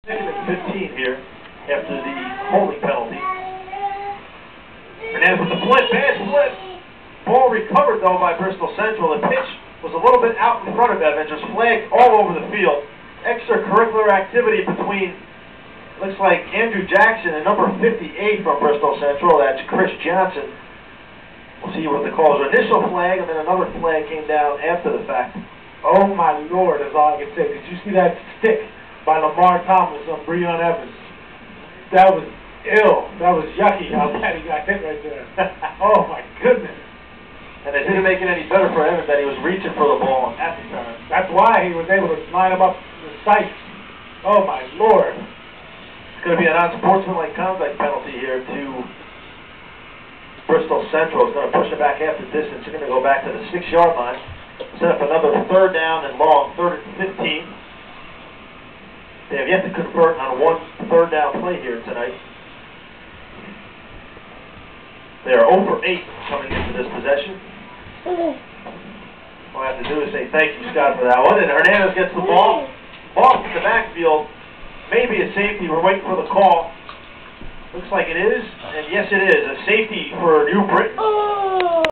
Text Book Tackling
# 30 Lamar Thomas New Britain High defensive end sticks top prospect Breon Evans running back Bristol Central